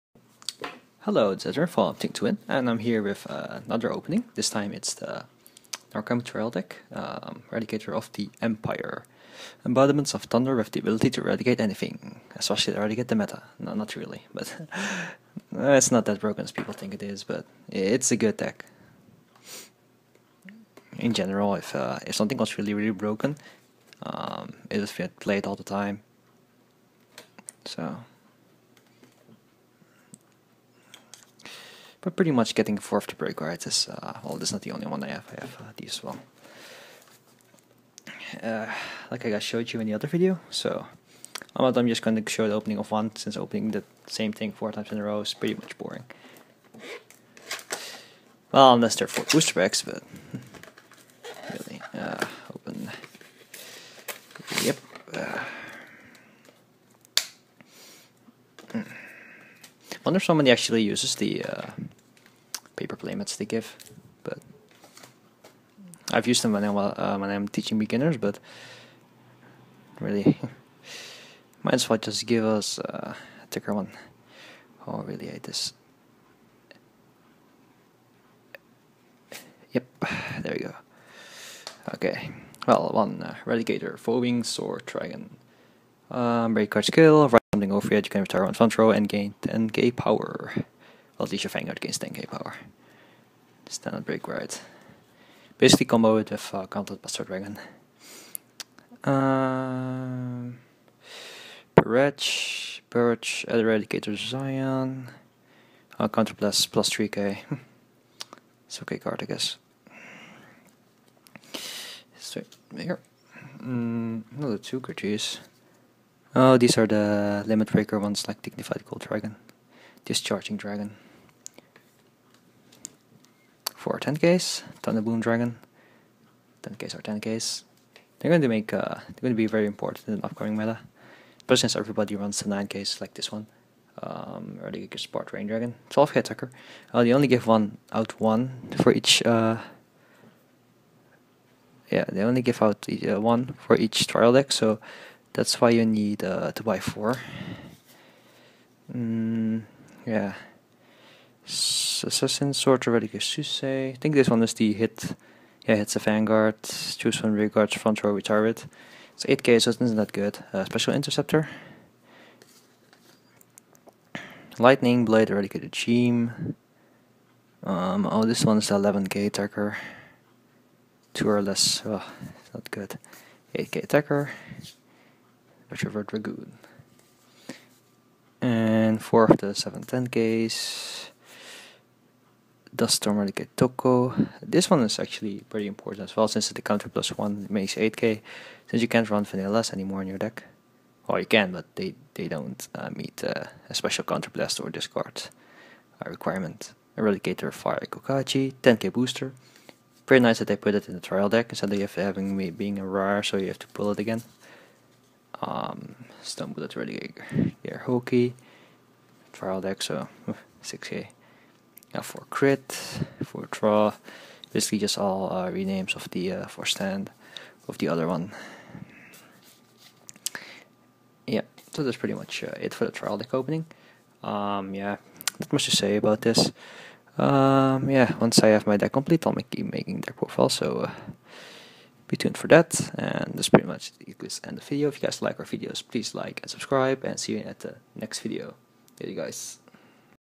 Cardfight!!! Vanguard TD09: Eradicator of the Empire Opening
In this video Edwin unboxes 1 out of his 4 Trial Deck 9: Eradicator of the Empire decks which he will use to make his upcoming Eradicator Deck.